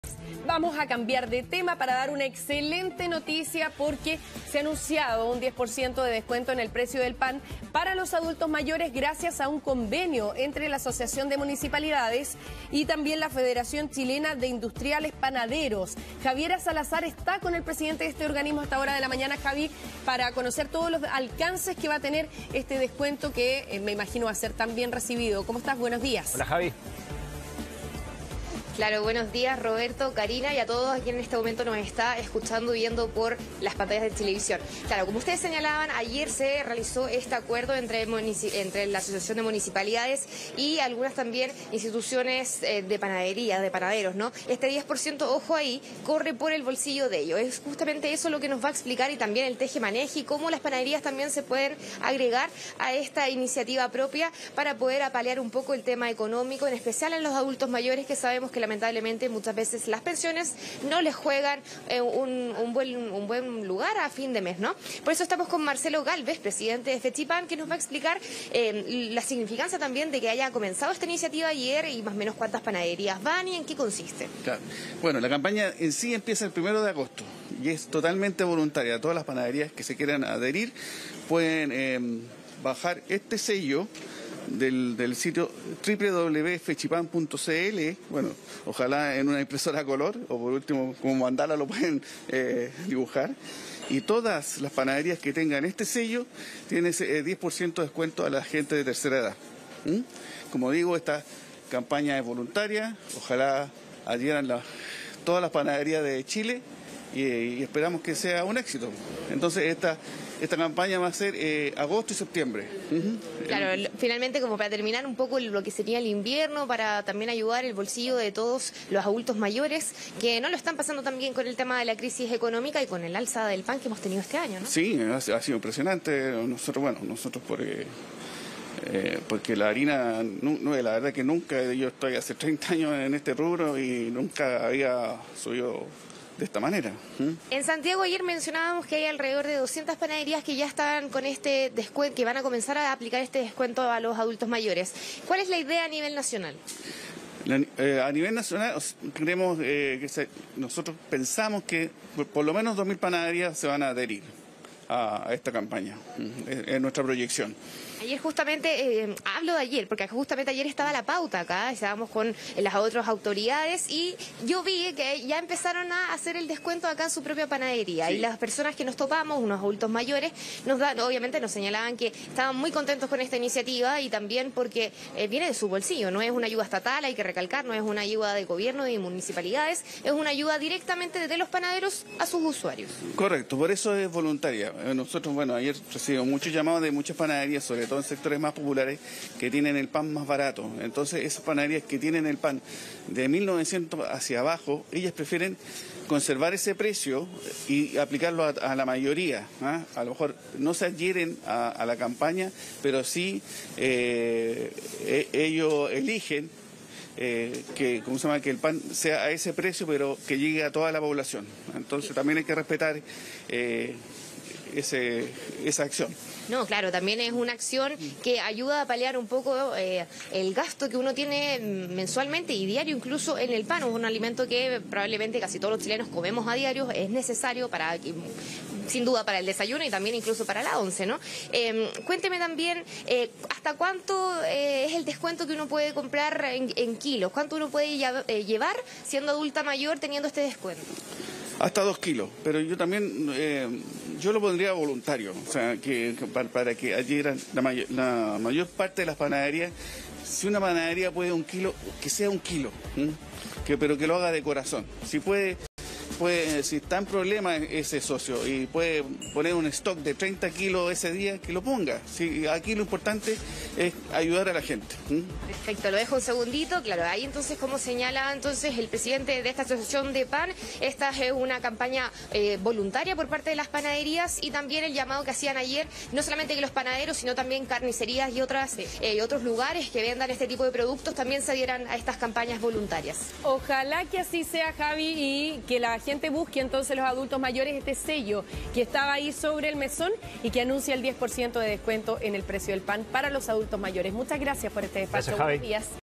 Descuento para adultos mayores en el precio del pan: ¿Qué hay que hacer para obtener el beneficio?
Los mayores de 65 años pagarán hasta un 10% menos del esencial alimento en las panaderías seleccionadas, iniciativa que se logró tras un acuerdo con la Asociación de Municipalidades de Chile.